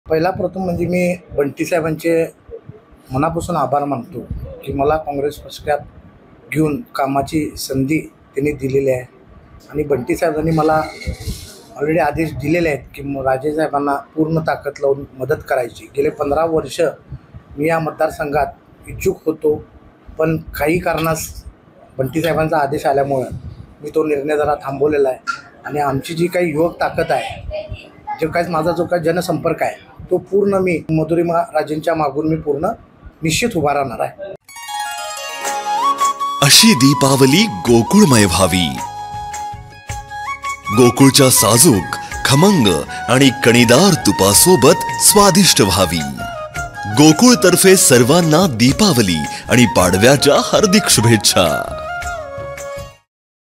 मला बंटी साहेबांचा आदेश आल्याने निर्णय बदलला आहे - दौलत देसाई
Marathi language.